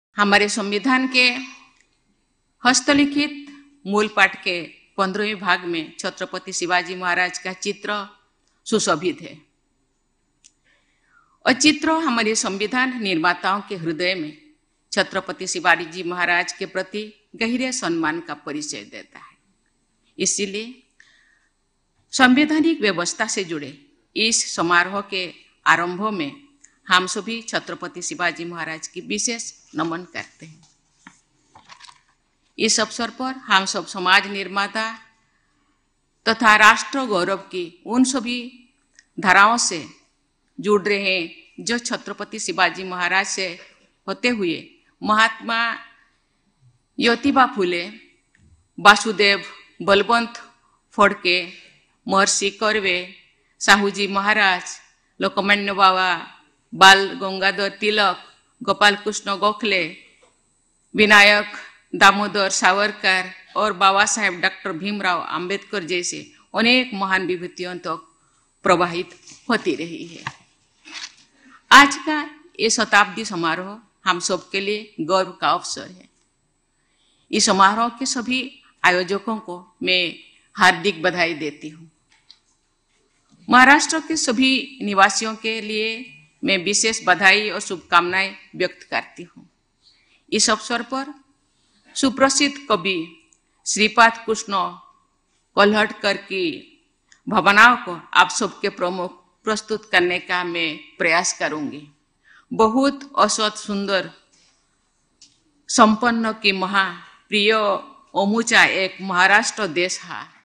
VidhanParishad | छत्रपति शिवाजी महाराजजी का चित्र संविधान में अंकित...|मुंबई
(महाराष्ट्र विधान परिषद शतक महोत्सवी कार्यक्रम | विधान भवन, मुंबई | 3-9-2024)